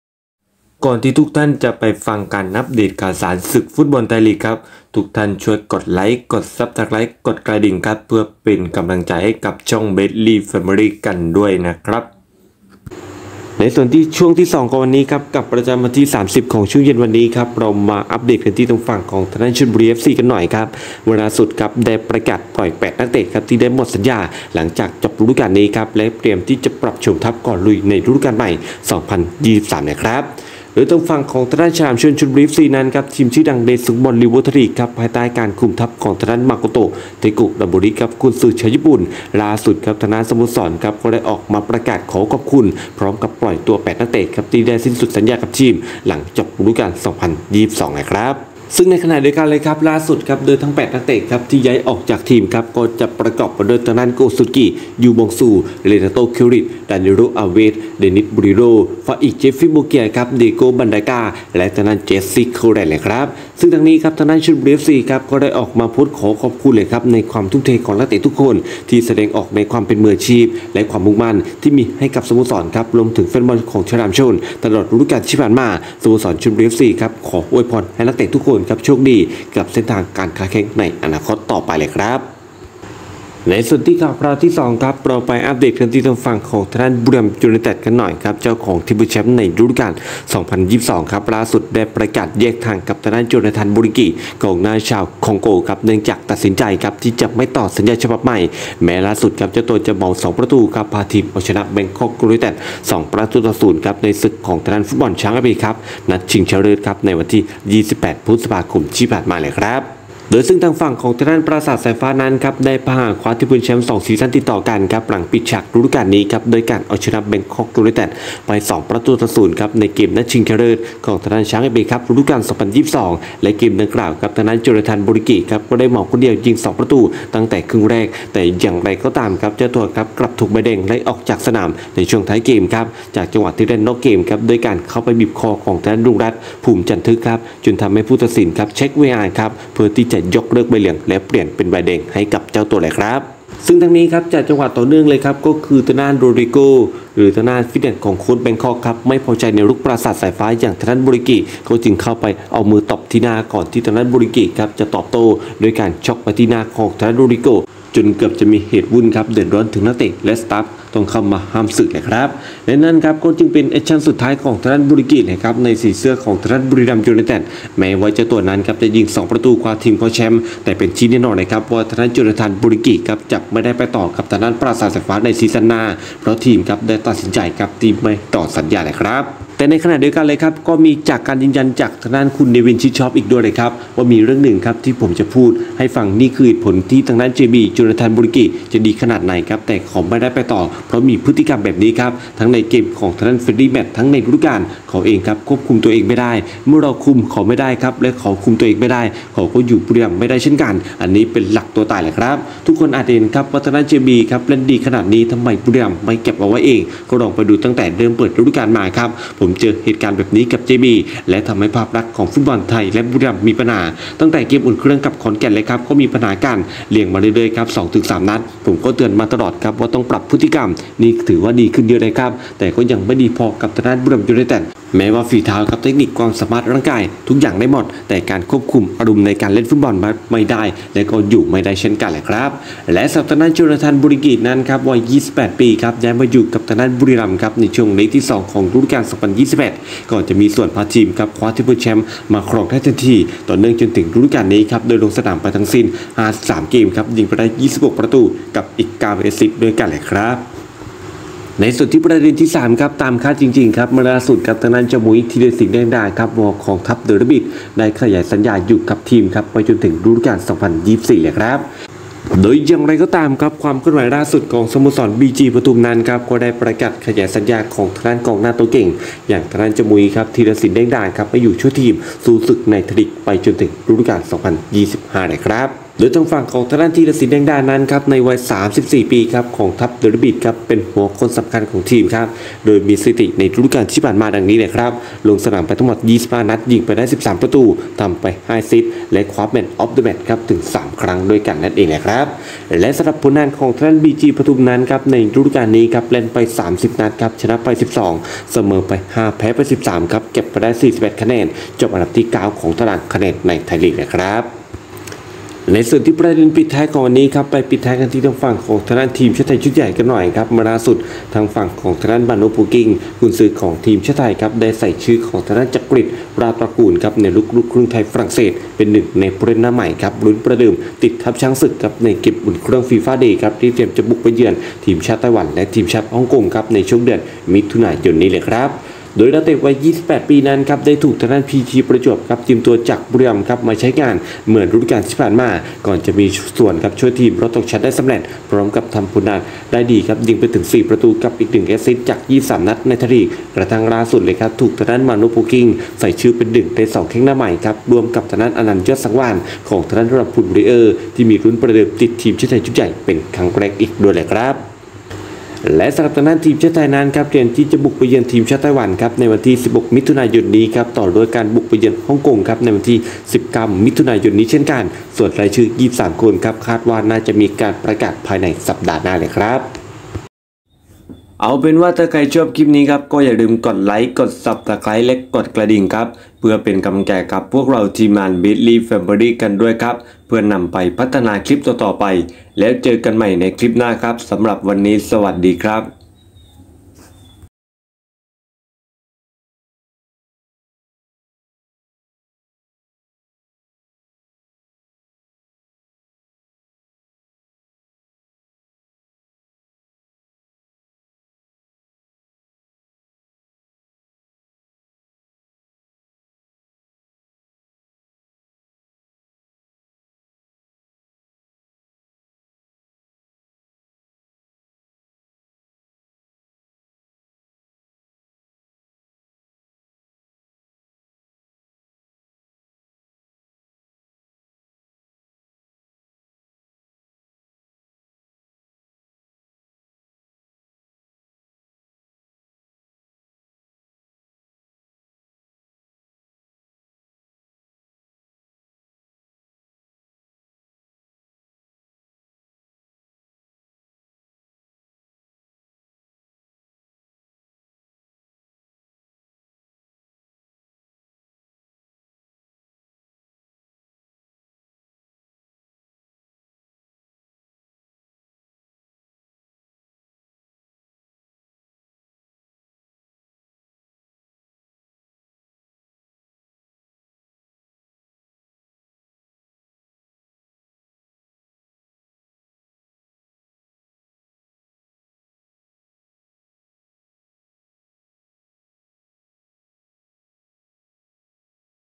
บุรีรัมย์ แยกทางกับโบลินกิ, ชลบุรี ปล่อย 8 นักเตะ, บีจี ปทุมฯ ต่อสัญญาธีรศิลป์
ประเด็นข่าวในวันนี้
1. บุรีรัมย์ แยกทางกับโบลินกิ
2.  ชลบุรี ปล่อย 8 นักเตะ
3. บีจี ปทุมฯ ต่อสัญญาธีรศิลป์
4. มาโน่ ใส่ชื่อจักรกฤษ ลุ้นติดธงช้างศึกชุดใหญ่

ติดตามเราได้อีกช่องทาง เพจ facebook : เบสลี่ Family V.2

เครดิตของข่าว